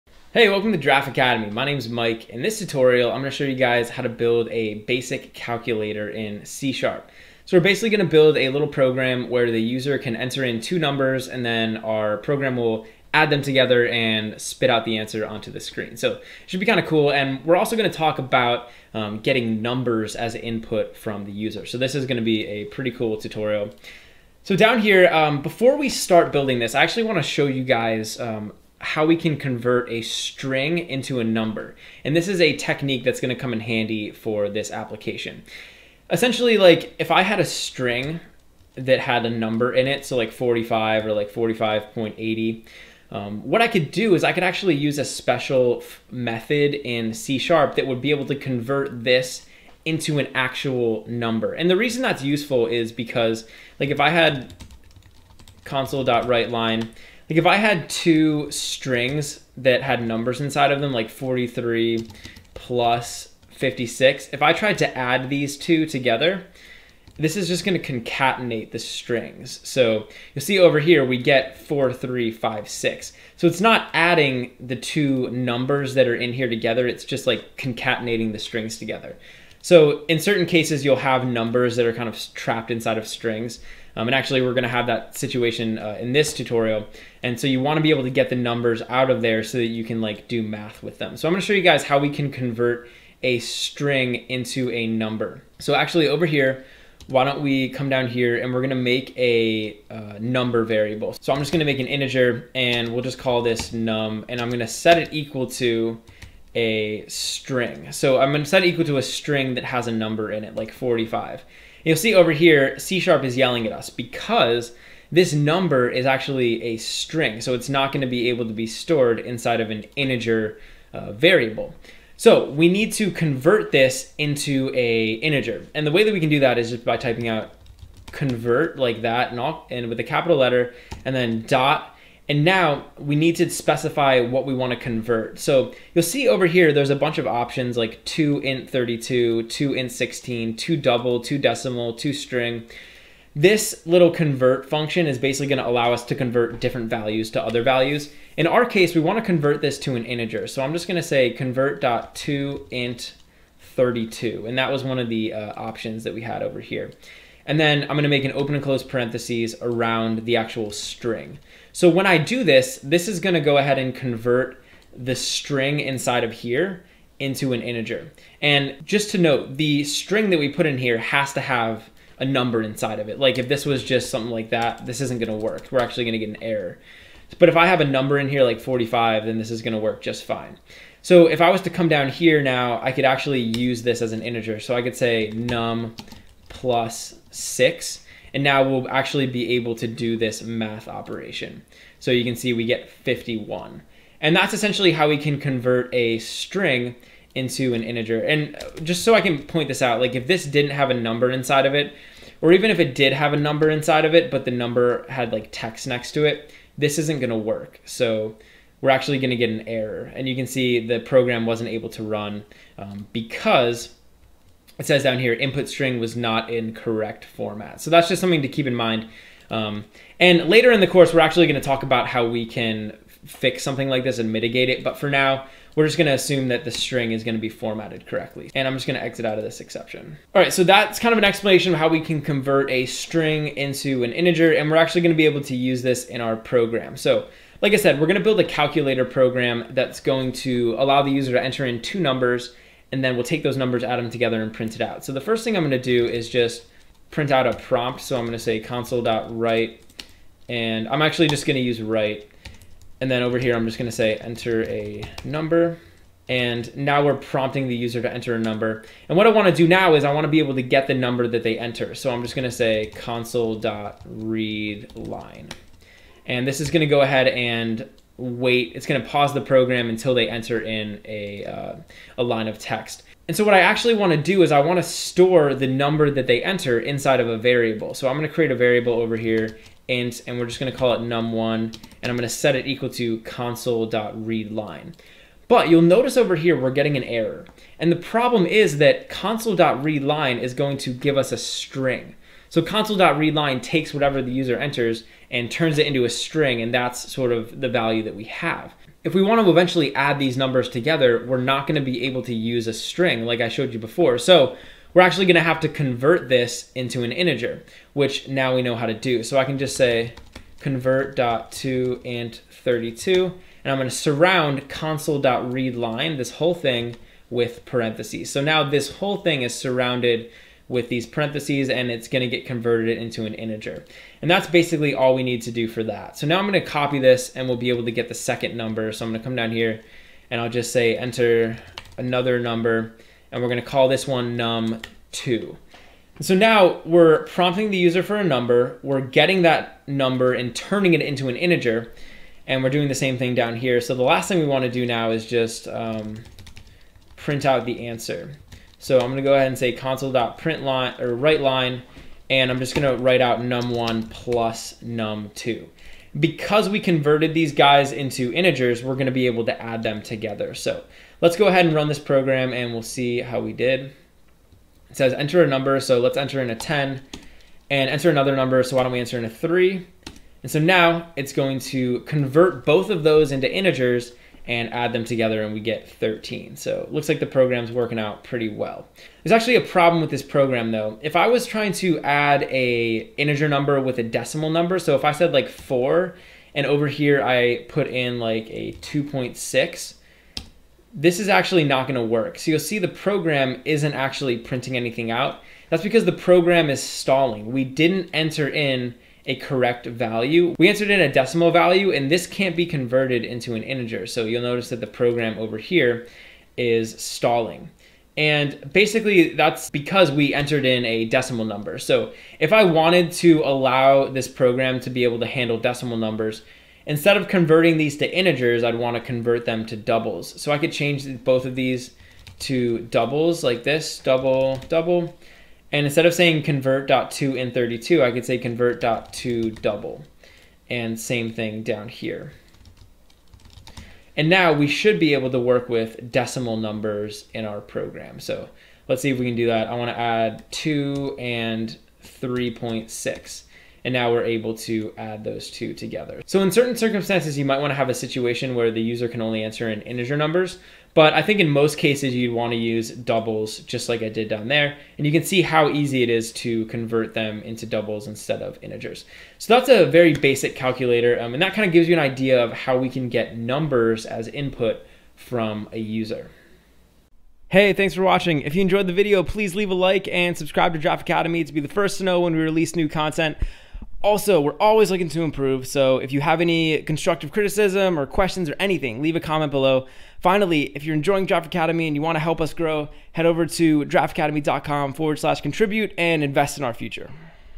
Building a Calculator | C# | Tutorial 9
This video is one in a series of videos where we'll be looking at programming in C#. The course is designed for new programmers, and will introduce common programming topics using the C# language.

If you’re enjoying Giraffe Academy and want to invest in our future, consider leaving a contribution